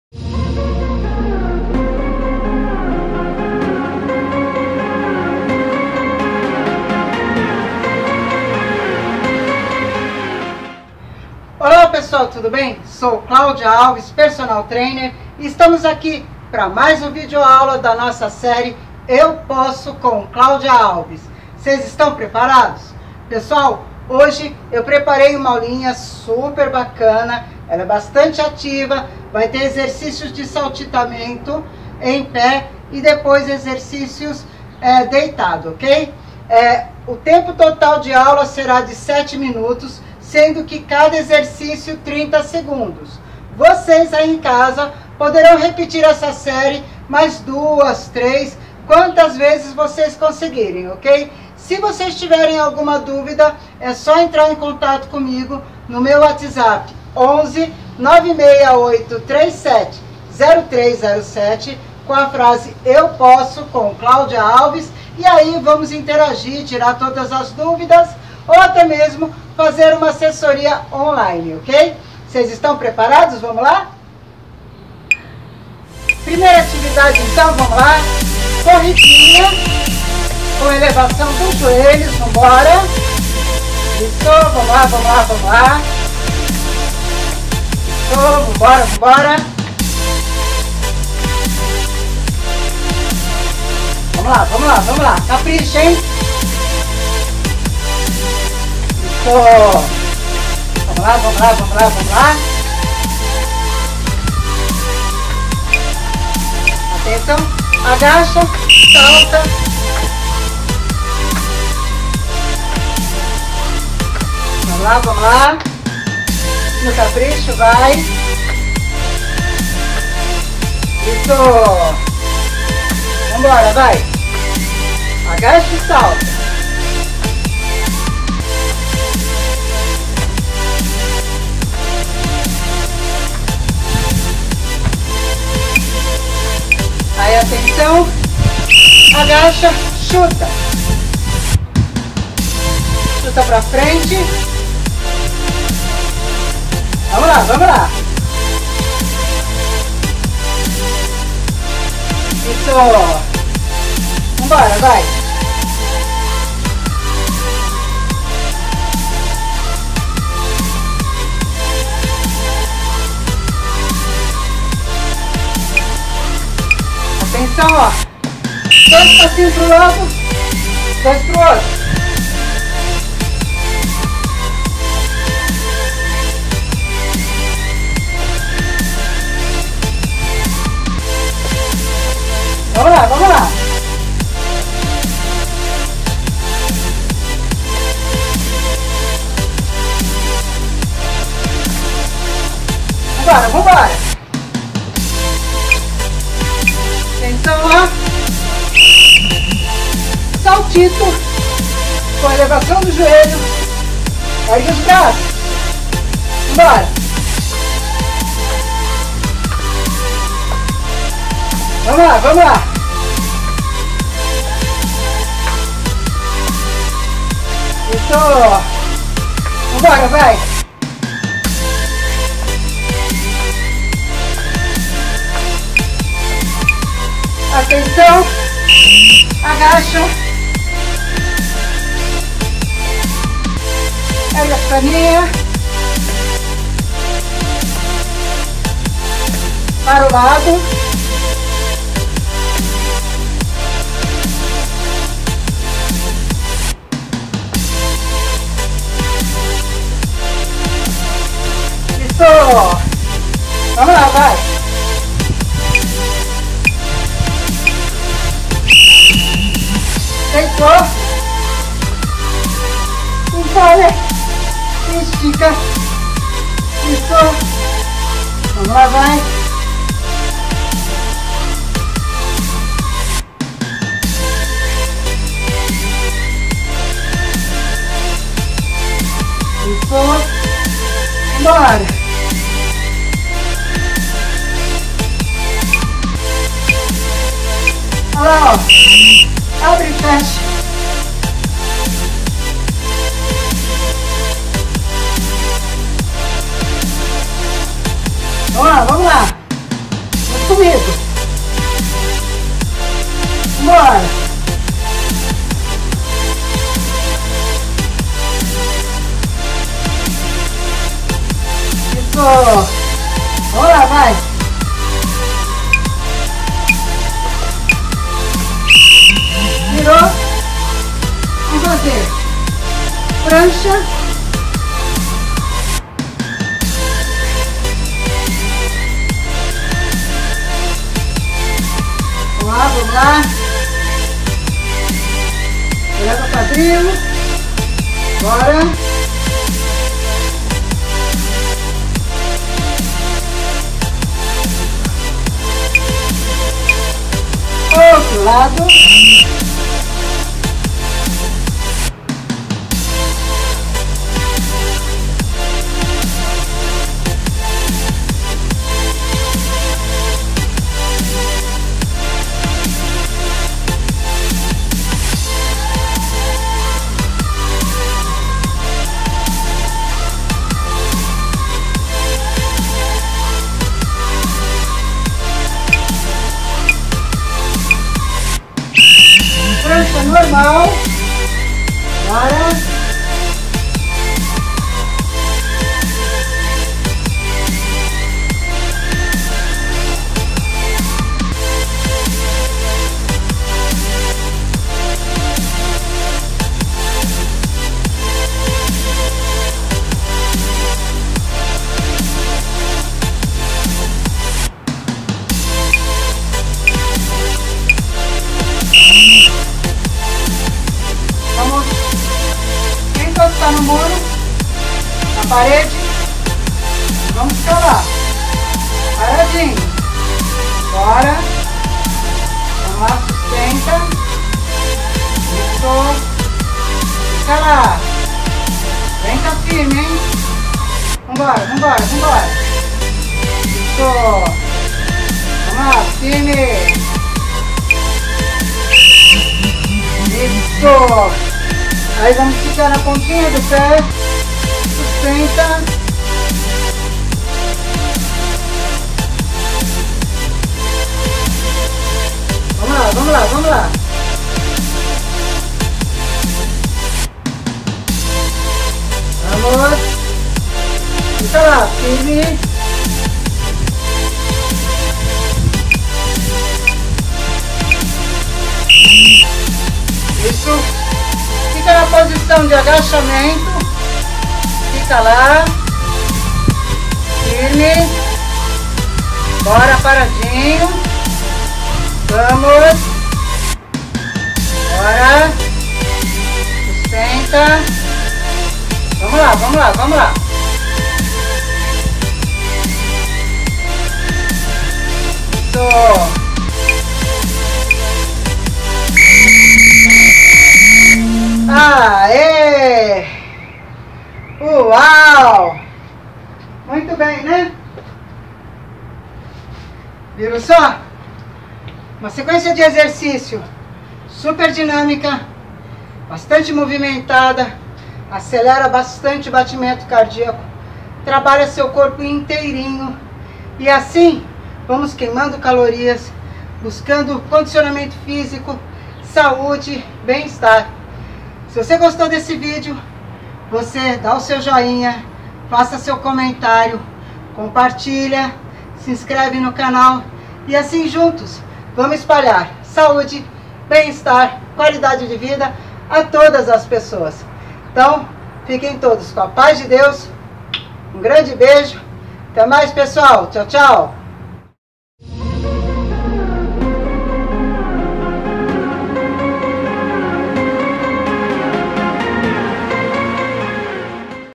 Programa Eu Posso Com Claudia Alves - aula 38
Hoje vamos com uma aula super top pra acelerar o batimento cardíaco.
Vocês estão preparados???
Siga também meu canal: Missionária Claudia Alves